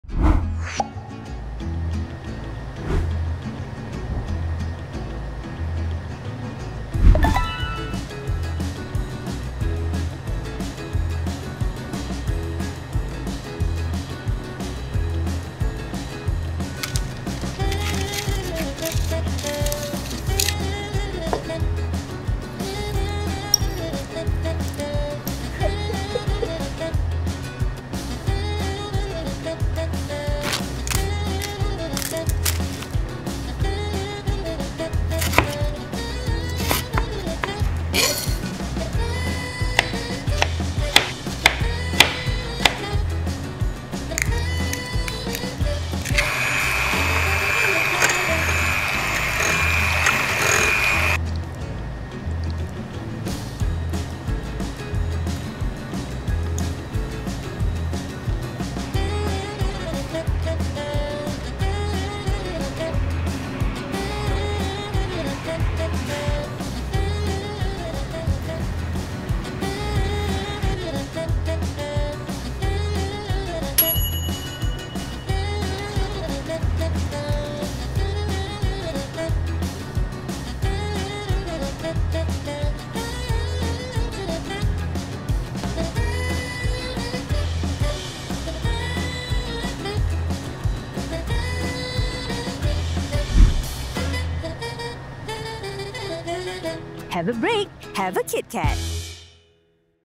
KITKAT Kek Pisang
Bersama-sama kongsi rezeki dengan KITKAT Kek Pisang! 🍌

Senang dimasak, waaaaaangi dan delicious sekali, Kek Pisang ini tentu akan meriahkan Raya anda!

Waktu Penyediaan: 15 min
Waktu memasak: 45 minit
Anggaran Hidangan: 9 keping

Dapatkan lebih banyak resipi dan inspirasi dengan KITKATEasyRecipeasy di halaman YouTube kami